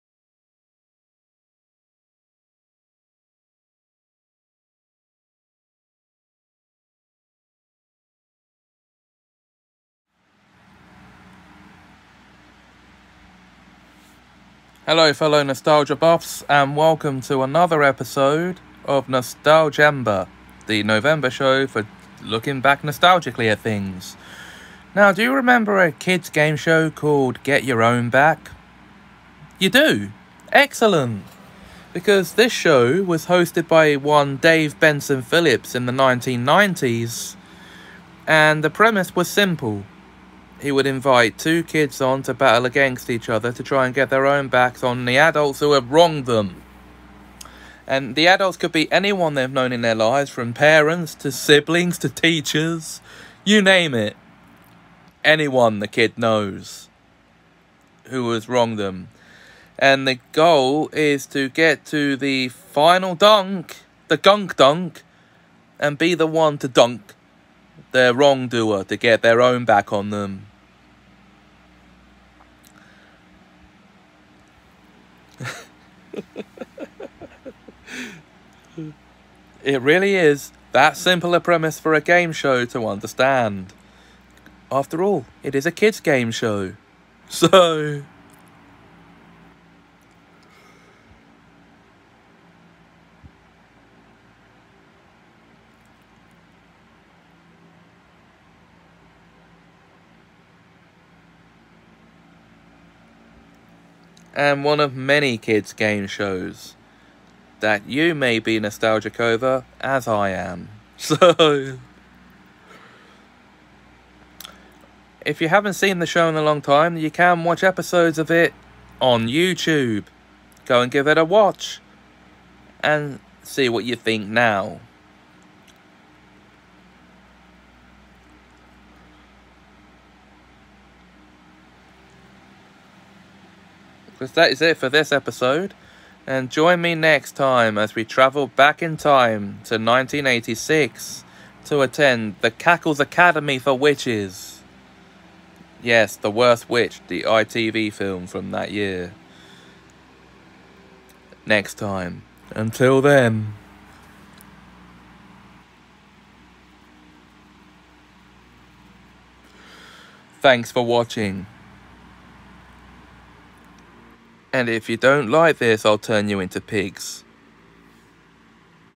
Nostalgiember No.27: Get Your Own Back
Nostalgiember: Series 2, Episode 6: I talk about the classic 90’s kid’s gameshow Get Your Own Back